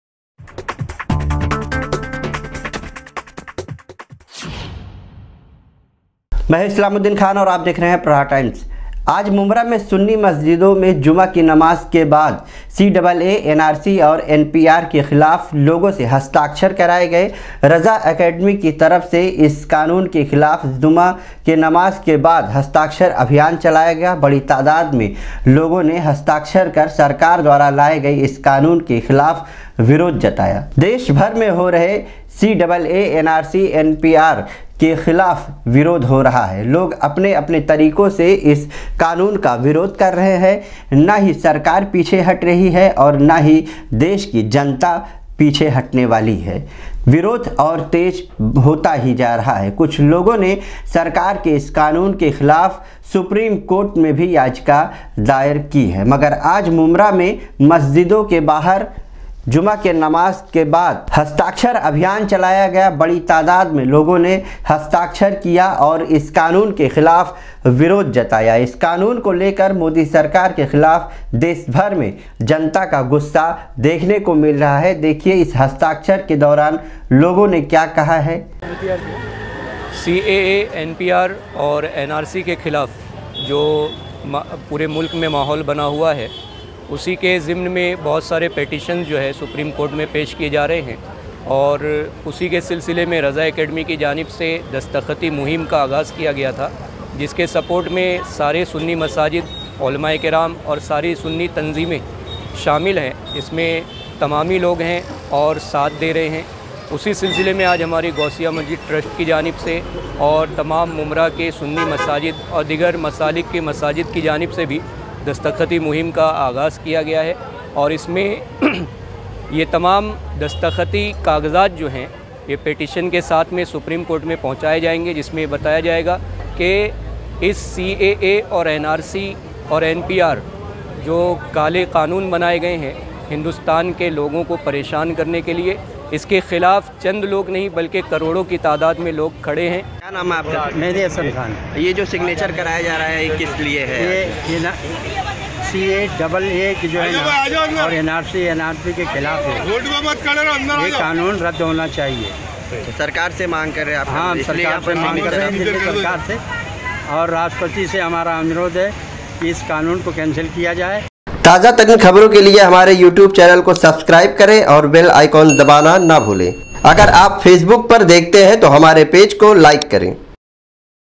Mumbra में जुमा की नमाज बाद CAA NRC के खिलाफ हस्ताक्षर||Islamuddin khan||Aapka Prahar Times
Mumbra में जुमा की नमाज बाद CAA NRC के खिलाफ हस्ताक्षर

CAA NRC NPR के खिलाफ हस्ताक्षर अभियान
जुमा की नमाज बाद लोगो ने किया हस्ताक्षर